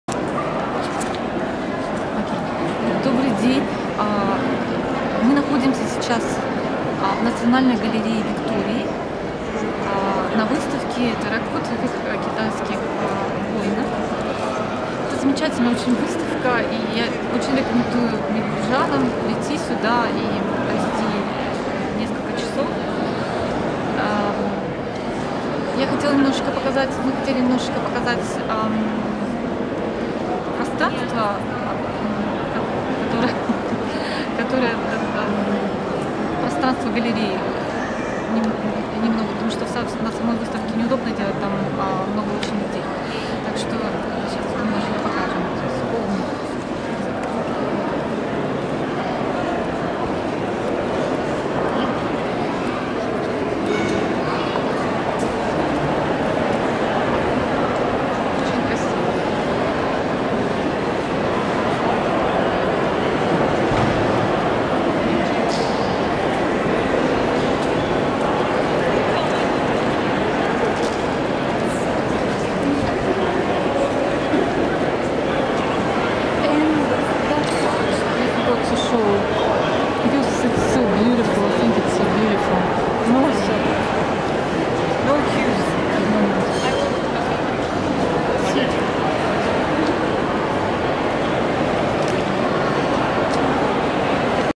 Мельбурн: Выставка китайских терракотовых воинов и работ Кай Гуо-Чанг
Выставка китайских терракотовых воинов и работ современника Кай Гуо-Чанг. Воины впечатляют, но и работы Гуо-Чанга (Cai Guo-Qiang) очень интересны.